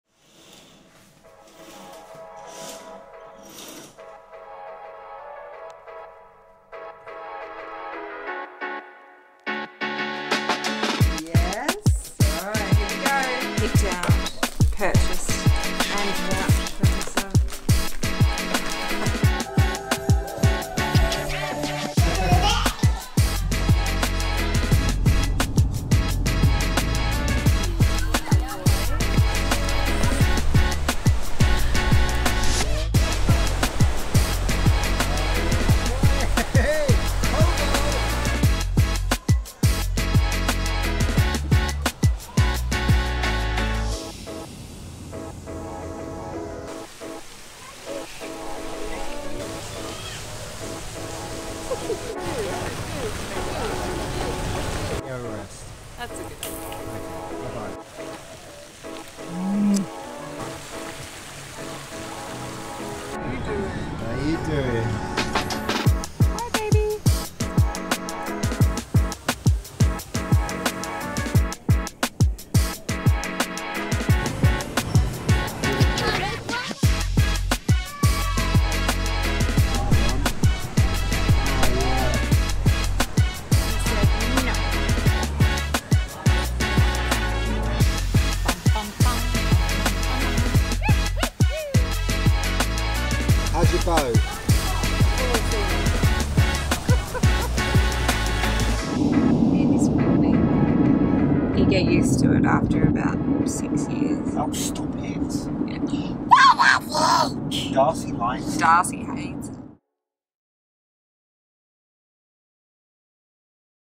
Melissa — Thank you for bringing the fun — Sony A7S3
Happy Birthday to my gorgeous wife, who always brings the fun to every occasion. Love ya x